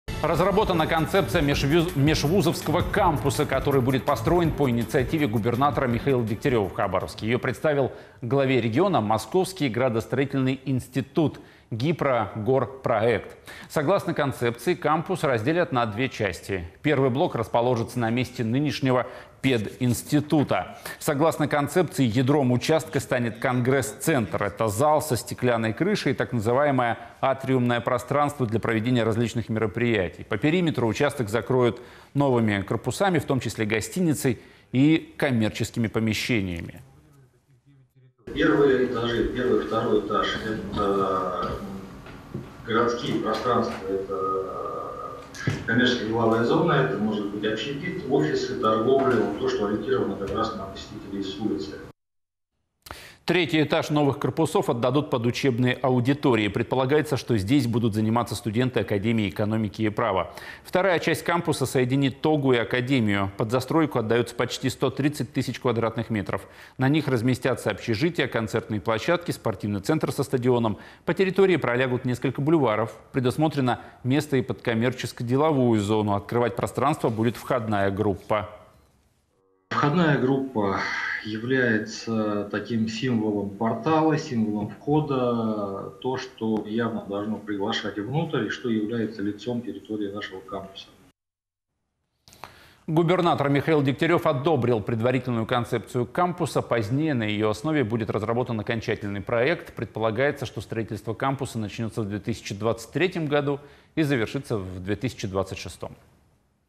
Межвузовский кампус начнут строить в Хабаровске в следующем году. Новости.28/01/22
В 2023 году в Хабаровске планируется приступить к строительству межвузовского кампуса – единого пространства научно-образовательных и социальных объектов для студентов, преподавателей и других жителей края. Проект обсудили глава региона Михаил Дегтярев и руководство ООО «Градостроительный Институт «Гипрогорпроект» в ходе рабочей встречи.